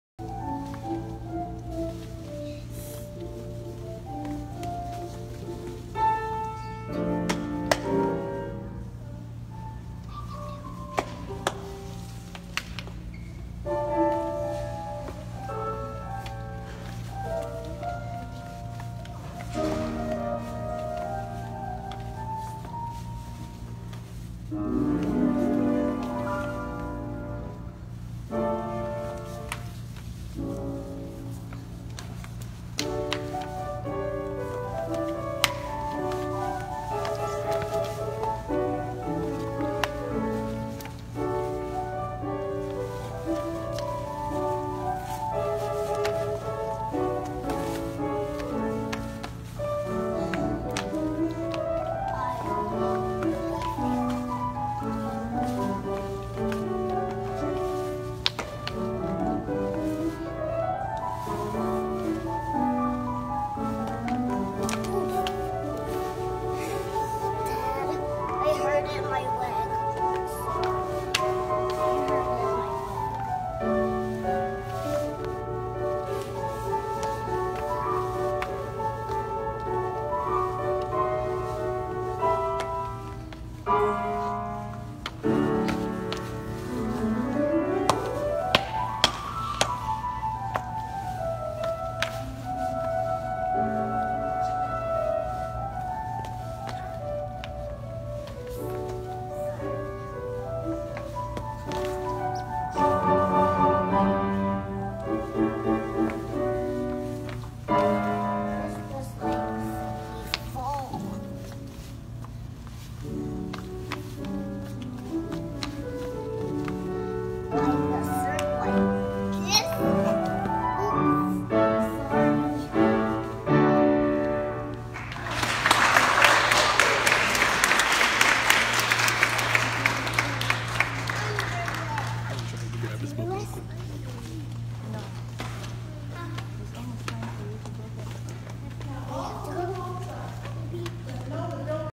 3yo piano players dancing backstage at recital - crazy cute
Vee and Mensah both started piano at Ms Ottley's School of Music in Hyattsville when they were both 2 years old. Here, they are celebrating the end of their 1st school year in piano lessons. They both got to dress up for the graduation recital. They both snuck  behind the pianos to have a private dance. Darling